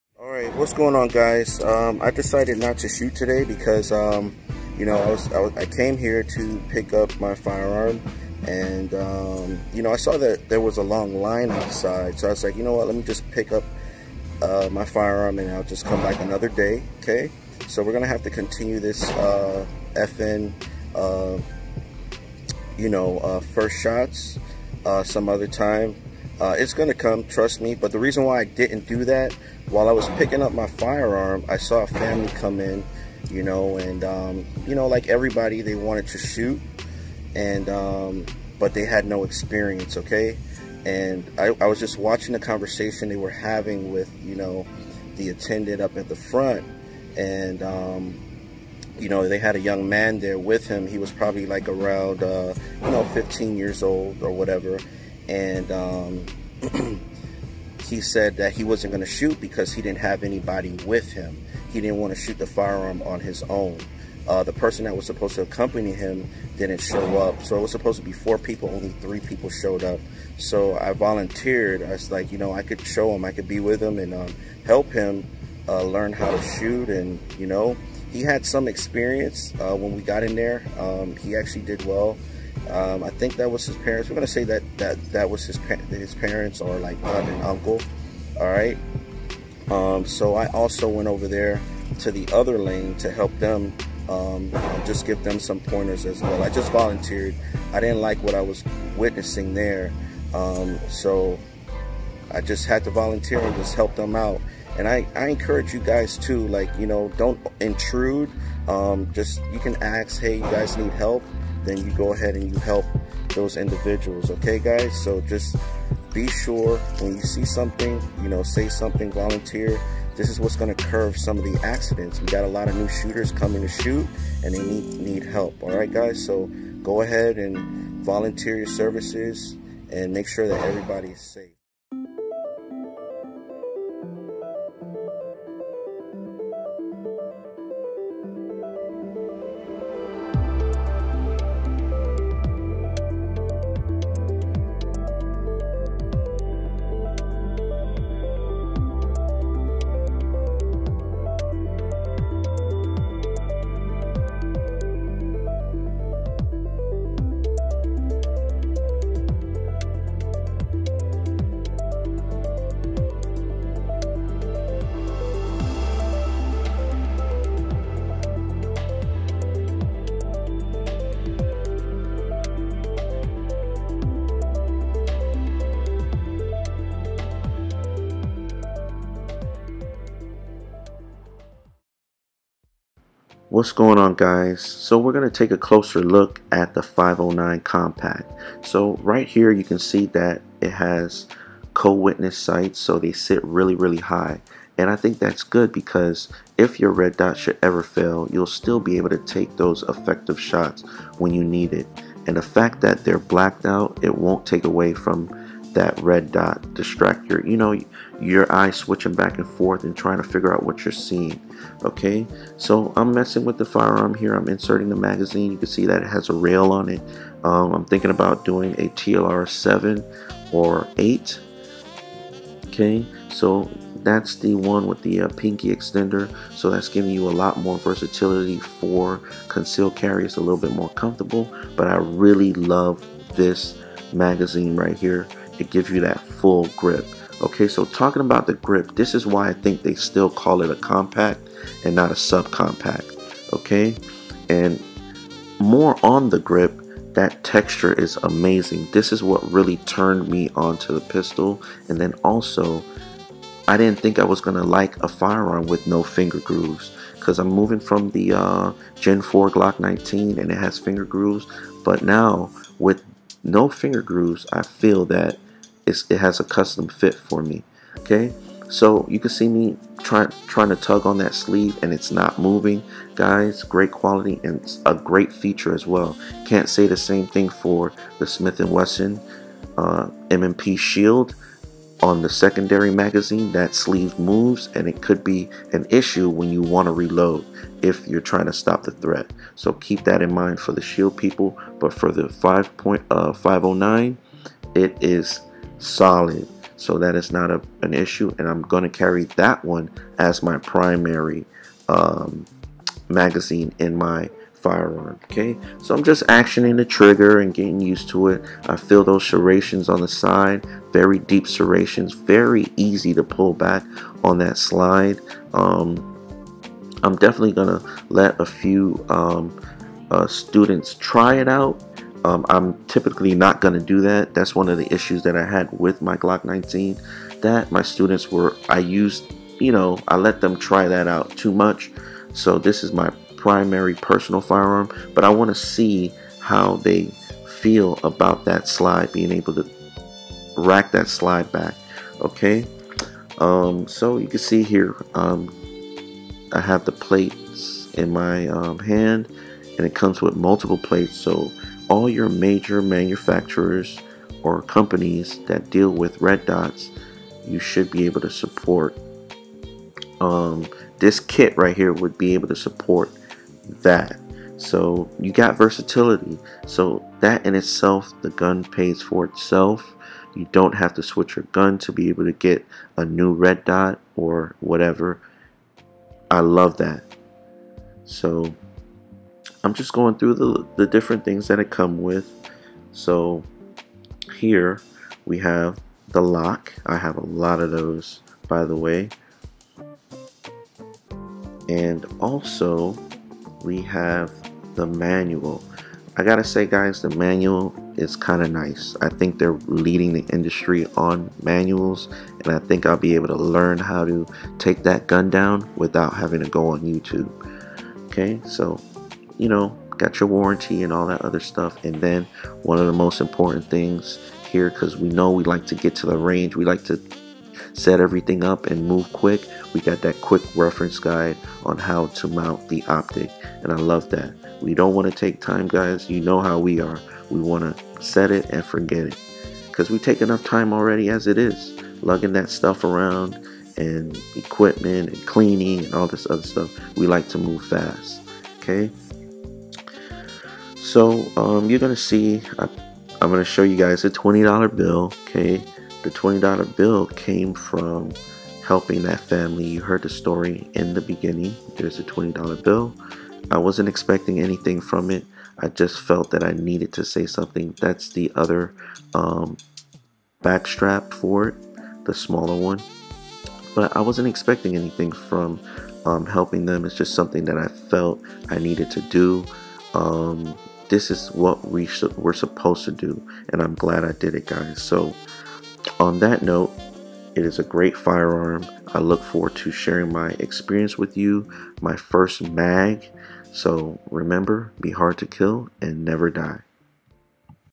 UNBOXING THE FN 509 COMPACT MRD BLK
Definitely excited I found the FN 509 compact! I search high and low. Truth be told I had my eyes set on the 509 midsize, but after holding the compact in my hand I was sold. check out my unboxing/ storytime/ closer look at the FN 509 compact MRD BLK.